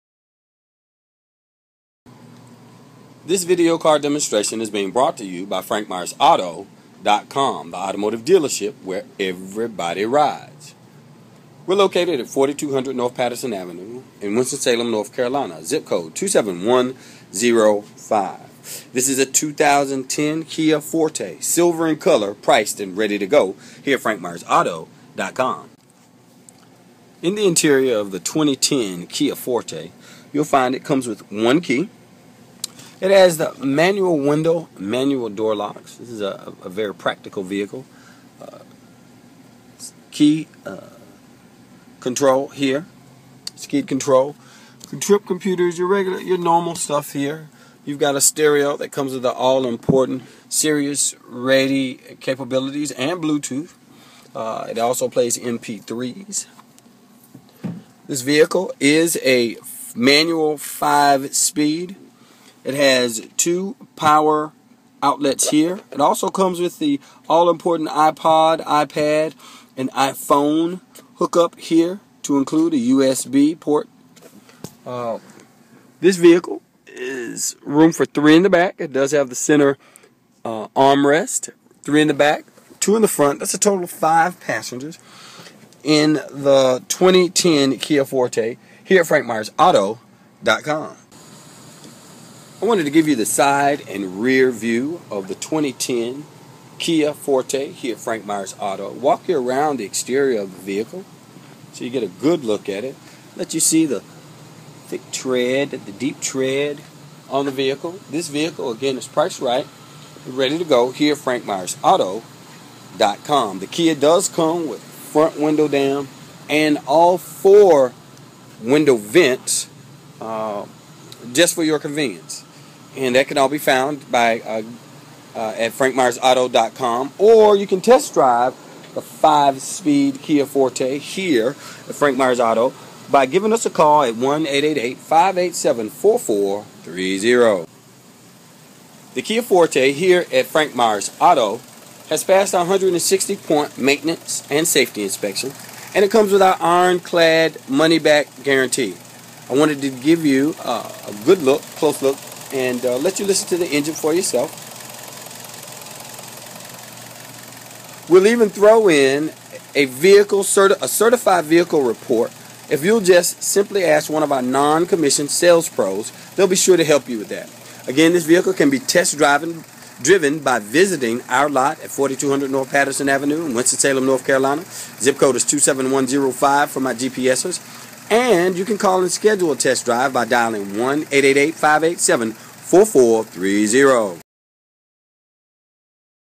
2010 Kia Forte 4dr Sdn Man LX 4 Door Car | Frank Myers
This is a 2010 Kia Forte 4dr Sdn Man LX 4 Door Car Front Wheel Drive with 5-Speed M/T transmission Silver[Bright Silver Metallic] color and Stone interior color.  This video is recorded and uploaded by cDemo Mobile Inspector.                             Detailed vehicle info with more photo(s) is available